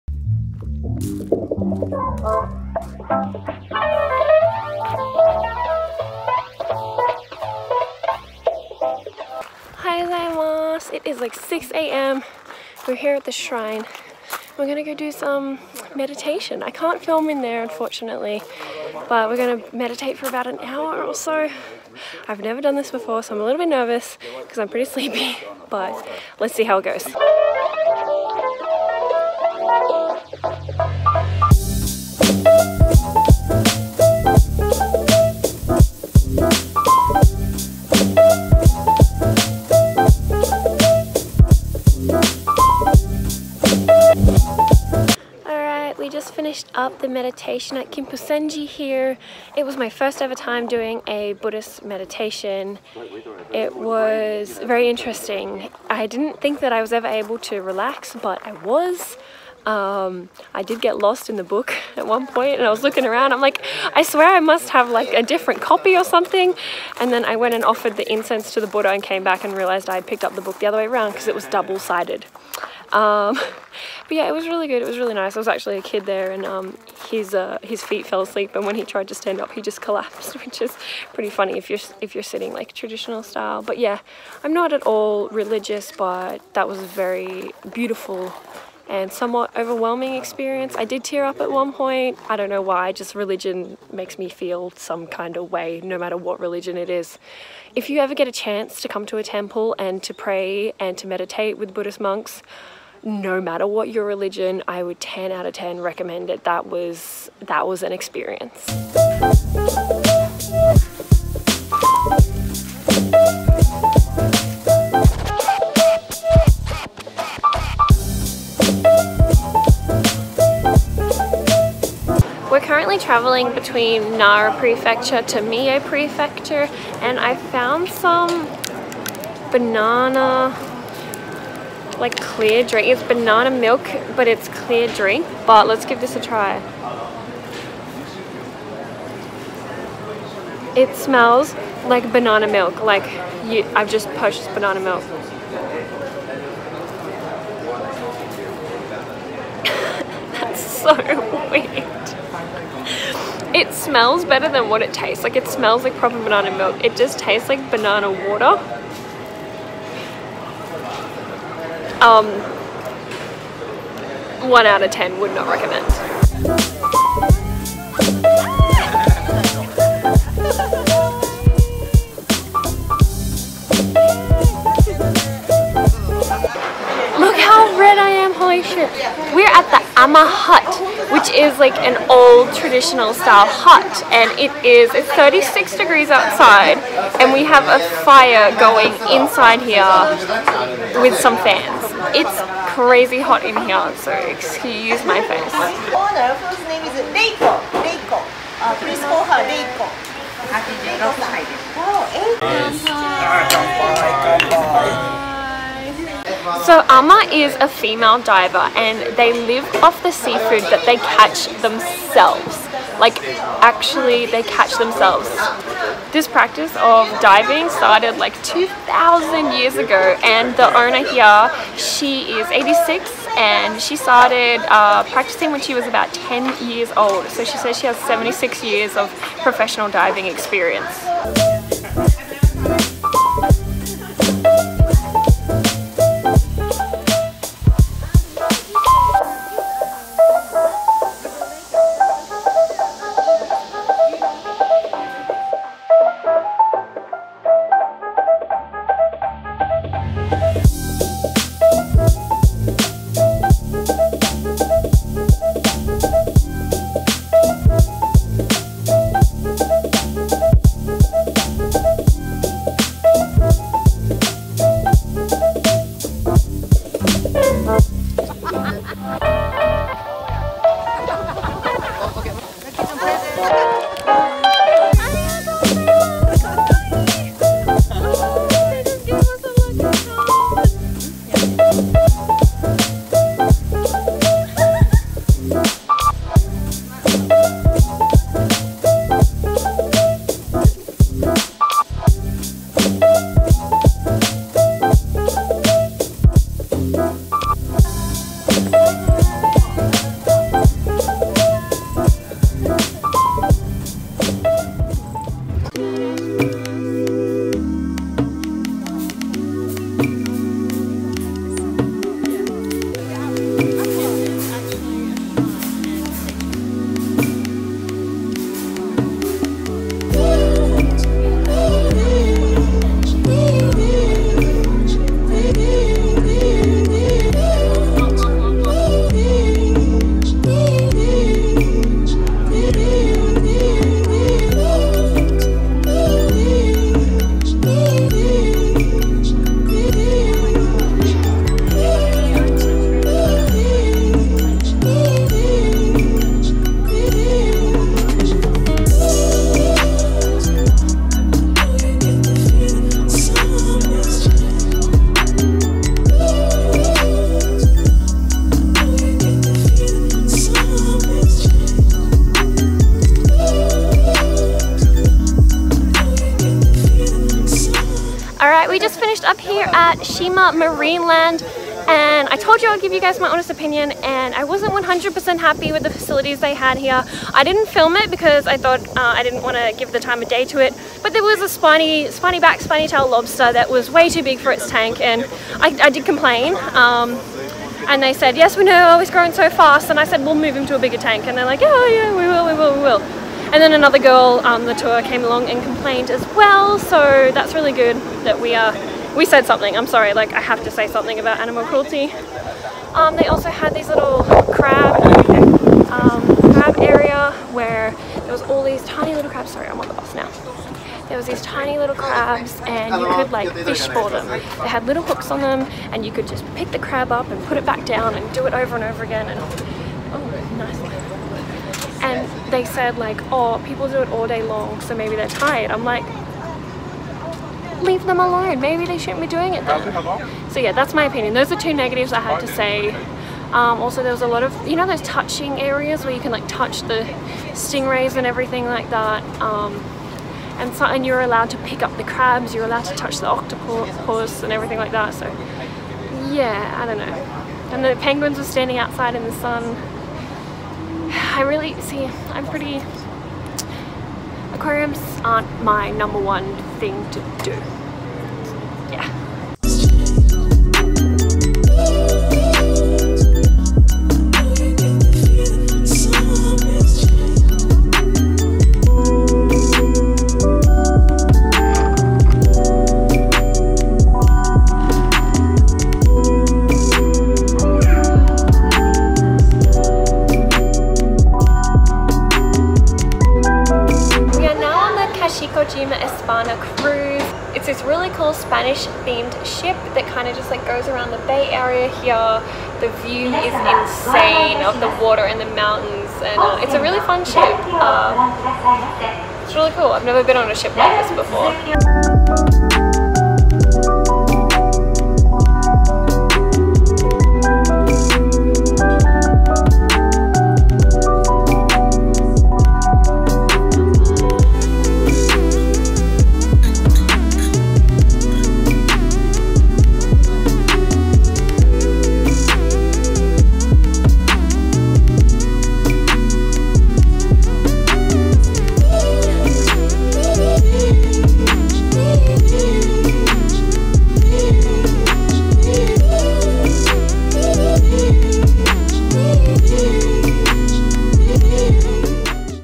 Toba & Shima🇯🇵 TRAVEL JAPAN
Join me as I start the day with some Buddhist meditation at Kinpusenji Temple, I tried some crazy clear Japanese banana milk, followed by visiting the Ama hut & divers. We then visited Shima Marineland and closed up the day on the Spanish boat Kashikojima Espana for a trip around Ago Bay followed by an almost $400 dinner!
*Filmed on iphone X*

My camera
My computer
My tripod
My Phone/Camera charger: "